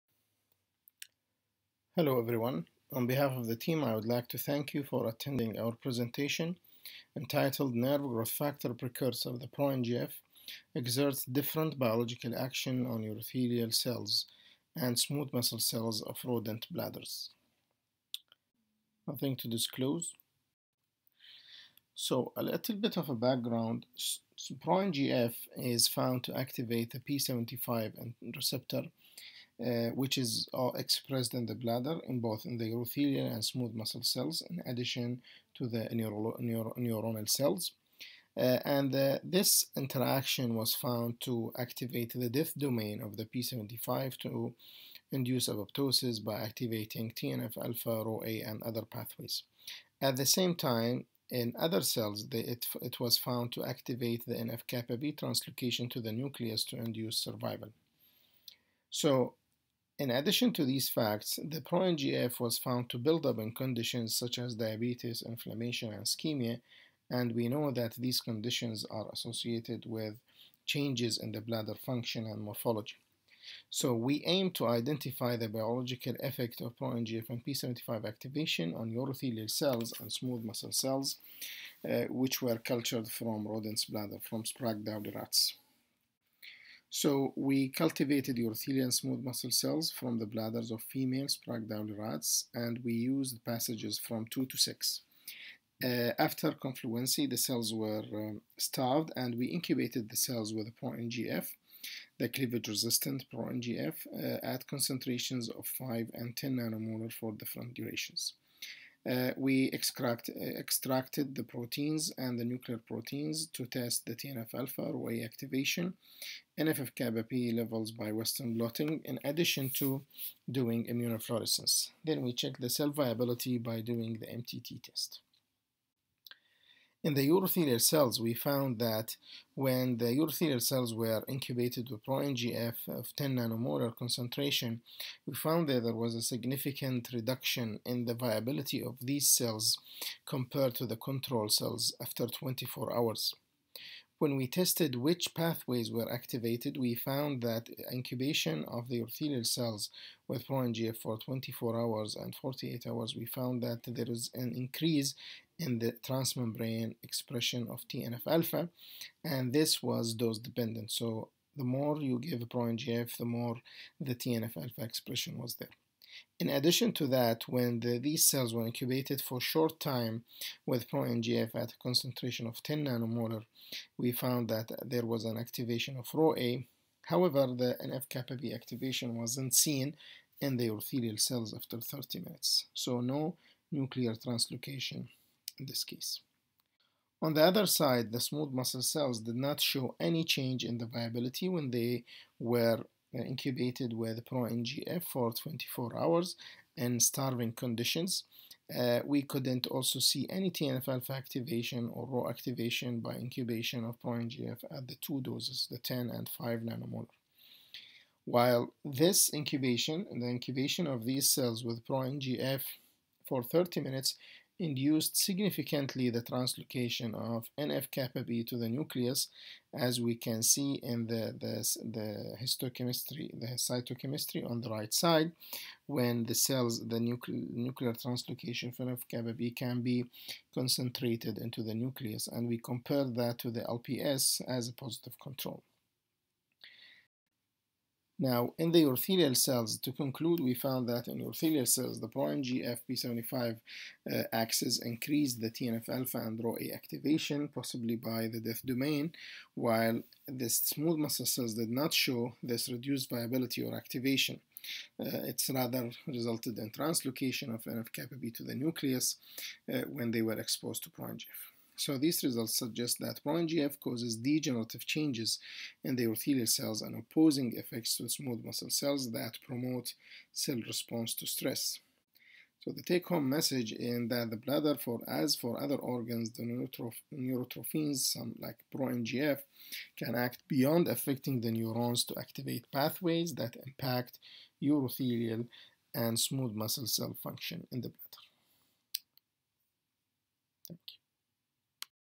#369 NERVE GROWTH FACTOR PRECURSOR (PRONGF) EXERTS DIFFERENT BIOLOGICAL ACTIONS ON UROTHELIAL AN...
NERVE GROWTH FACTOR PRECURSOR (PRONGF) EXERTS DIFFERENT BIOLOGICAL ACTIONS ON UROTHELIAL AND SMOOTH MUSCLE CELLS OF RODENTS BLADDERS.

Mossa A1, Cammisotto P2, Campeau L3

1. McGill University, 2. Lady Davis Institute, 3. Jewish General Hospital, McGill University

The nerve growth factor precursor (proNGF) activates p75NTR receptor and promotes cell death and degeneration in different tissues. Tissue levels of proNGF build up in conditions such as diabetes, inflammation and ischemia. Given that proNGF and p75NTR are expressed in different layers of the bladder, we aimed to identify the biological effect of proNGF/p75NTR activation on urothelial (UT) and smooth muscle (SM) cells of rodents’ bladder.